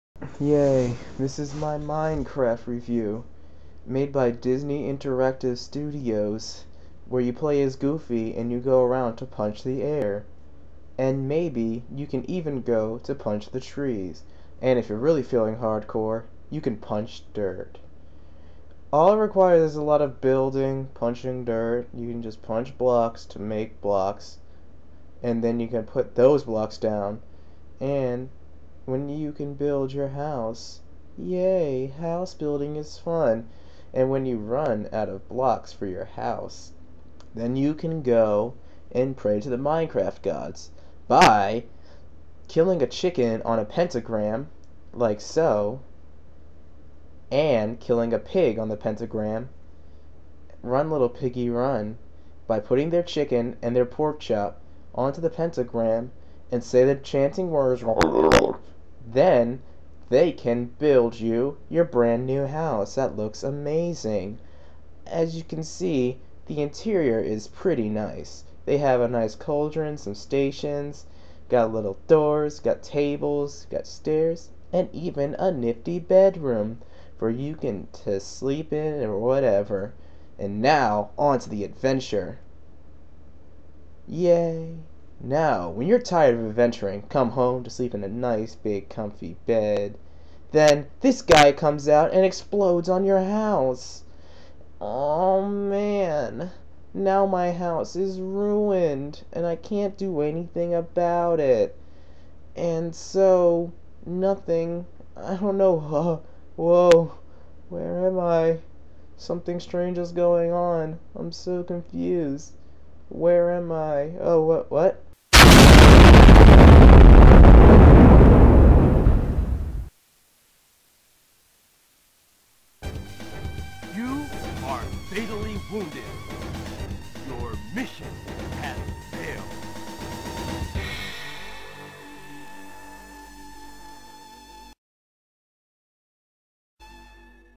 Minecraft Review
I really dropped the Ball on this review. No title credits, nor music. Either way it's rather witty.